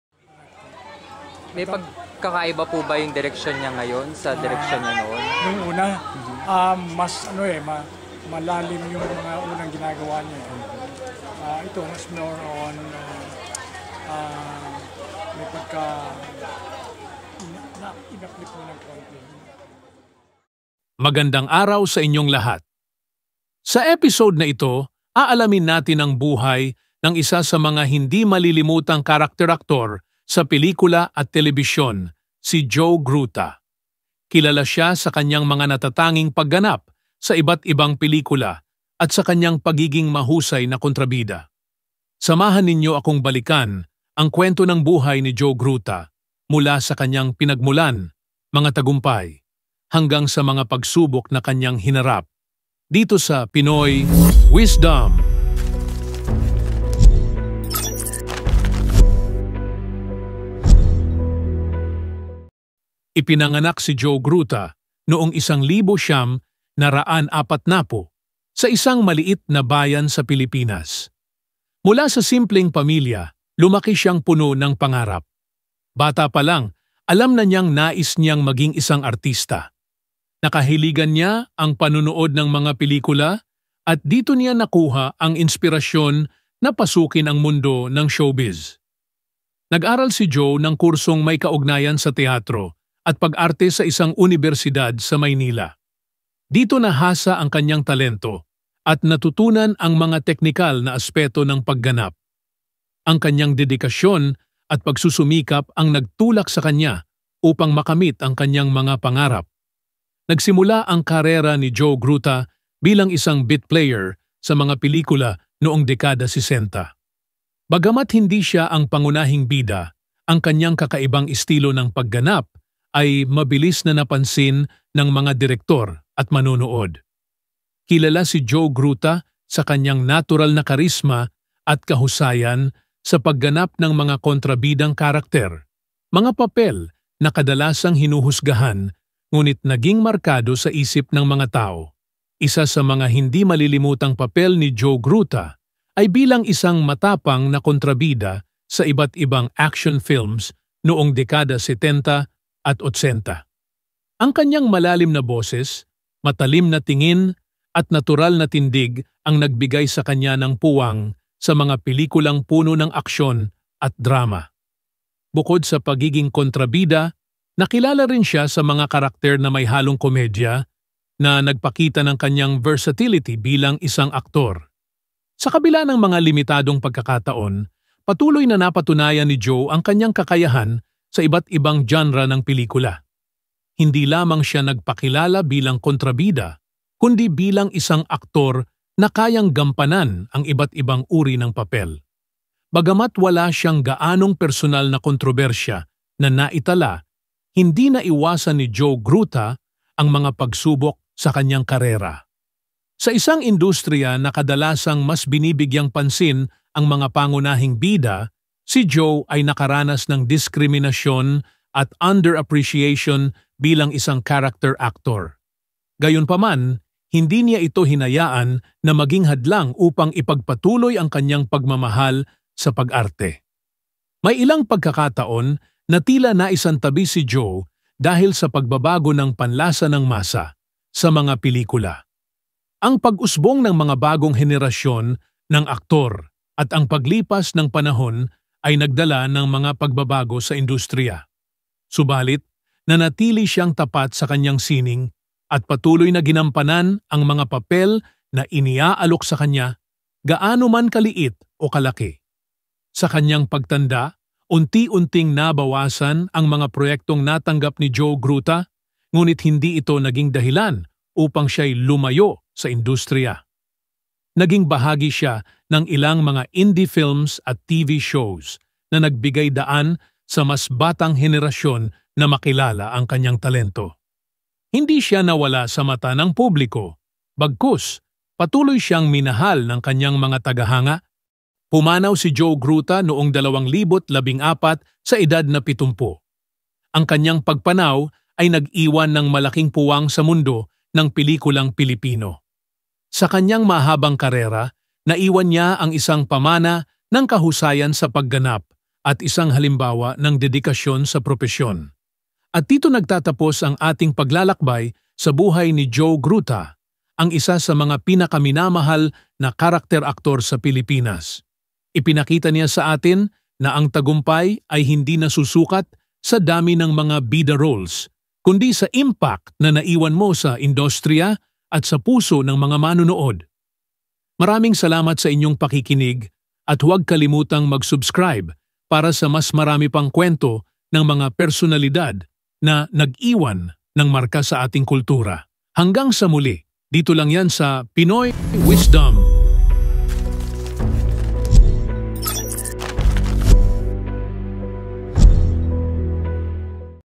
ANO NGA BA ANG NANGYARI KAY JOE GRUTA???
Joe Gruta, born on December 19, 1944, in Juban, Sorsogon, was a veteran Filipino actor known for his roles in both theater and film. He gained prominence in the 1970s, starring in films such as *A Dangerous Life*, *Juan Tamad at Mr. Shooli: Mongolian Barbecue*, and *Mumbaki*. Gruta worked with esteemed directors like Lino Brocka and Ishmael Bernal, and his acting extended into television series, including roles in *Maria la del Barrio* and *The Trial*. He was also a pioneer member of the Philippine Educational Theater Association (PETA). Joe Gruta passed away on March 10, 2024, at the age of 79.